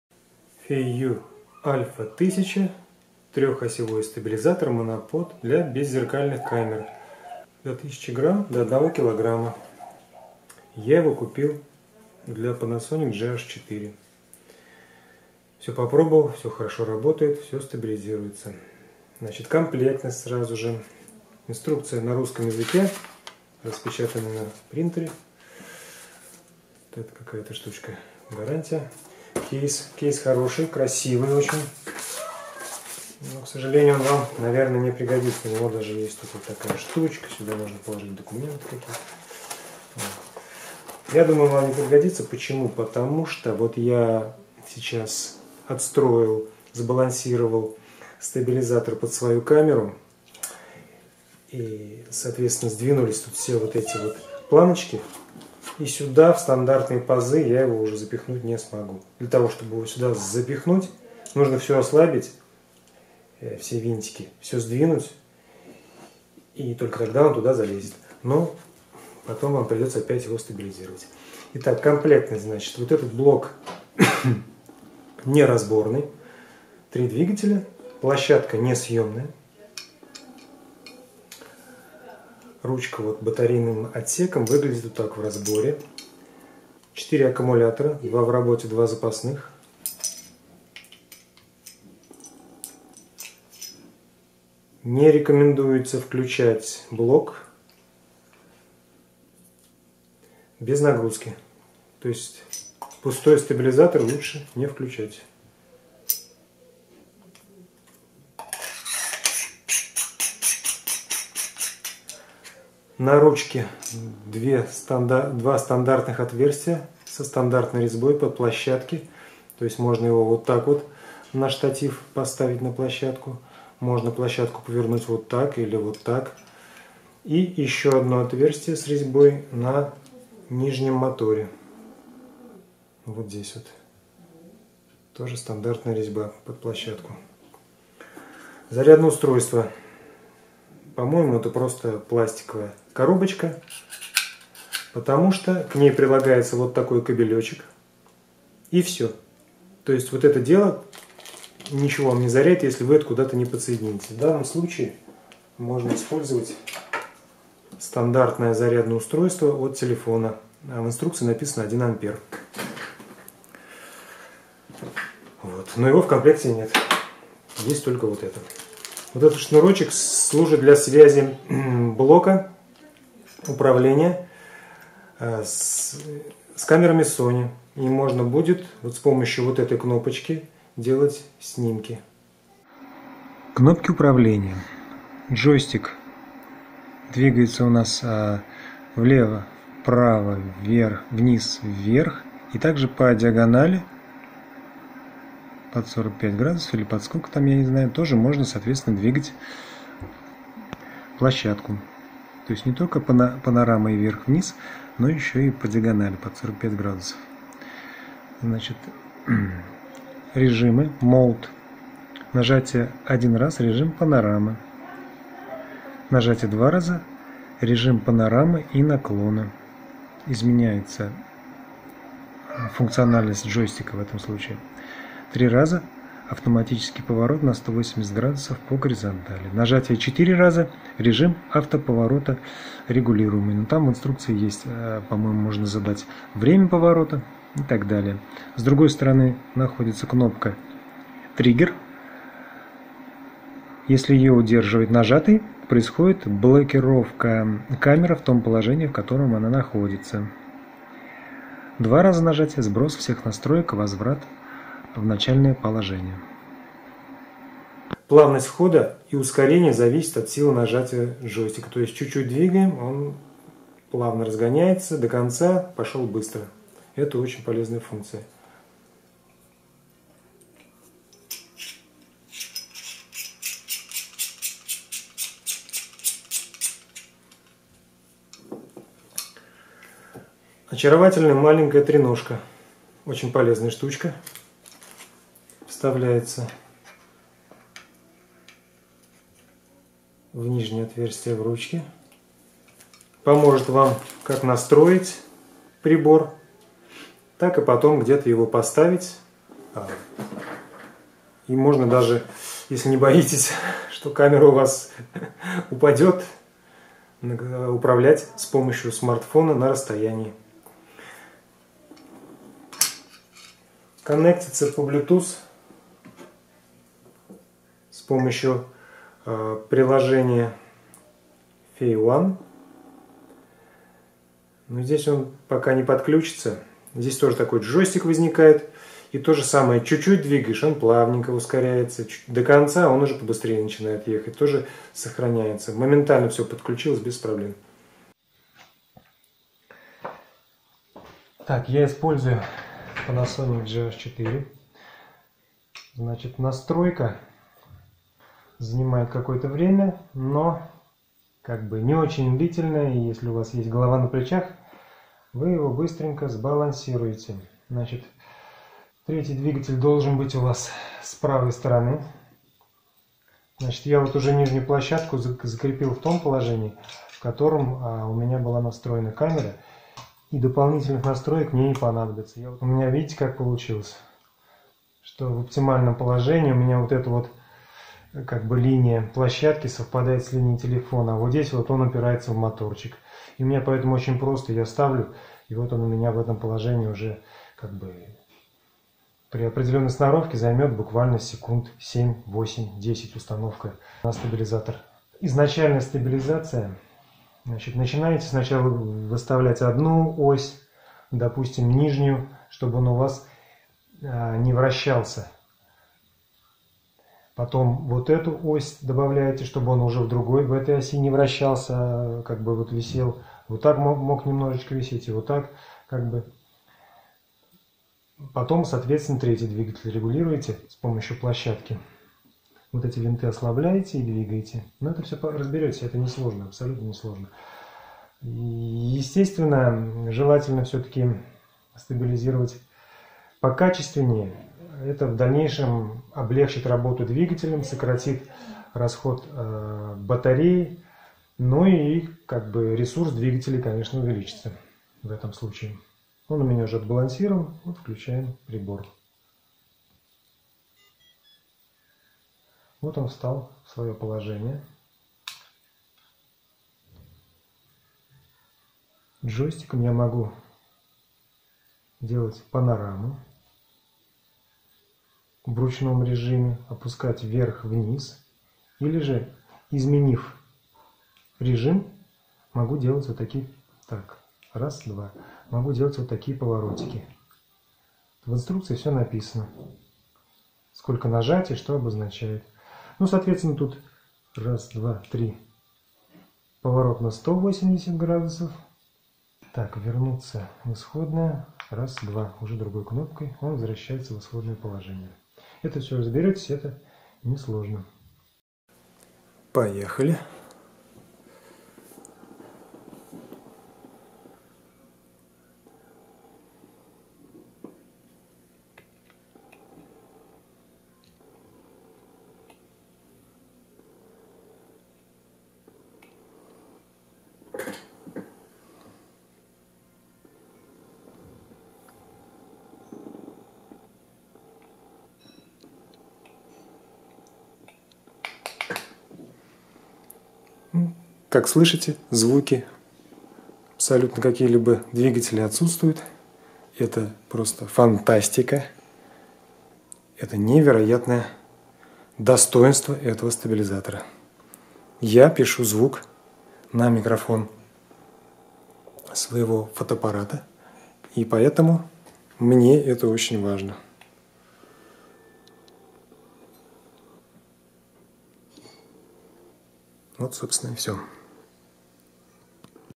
Feiyu Tech a1000 (старая версия) обзор
Внимание! В этом обзоре описана модель купленная в январе 2018. Есть информация, что в более новых версиях начиная с 06,2018 добавлена быстроснимающаяся  площадка и грузоподъёмность была увеличена до 1,7 кг. Будьте внимательны!
Сделан качественно, выглядит презентабельно. Плавный ход двигателей при управлении джойстиком. Почти бесшумный!!! Скорость поворота зависит от силы нажатия джойстика. Моментально коннектится по блютуз. С телефона так же можно управлять скоростью поворота. Наличие в комплекте маленькой треноги. 4 мощных аккумулятора. Два в работе, два запасных. Наличие стандартной резьбы для крепления к штативу:снизу в ручке, сбоку в ручке, на нижнем моторе.

Несъемная площадка. Хотя установка камеры на сбалансированный стабилизатор не занимает много времени. Можно запомнить или отметить маркером положение камеры, и ставить ее на это же место. Крепится одним винтом. Сбалансированный под вашу камеру стабилизатор вы вряд ли уложите в кейс в стандартные пазы. Придется ослабить винты и сдвинуть моторчики. Либо вырезать перегородки)) Нестандартные аккумуляторы. Хотя не дефицитные. Управление записью только для камер сони. У меня гаш, хотелось бы тоже). Зарядное устройство без вилки))) только шнур usb. походит 1 амперная зарядка от телефона с usb разъемом.

Комплектация: кейс, блок моторов с площадкой, ручка с управлением и батарейным отсеком, тренога, винт для площадки, кабель для sony, кабель для зарядного устройства, зарядное устройство, 4 аккумулятора, инструкция, гарантийный талон.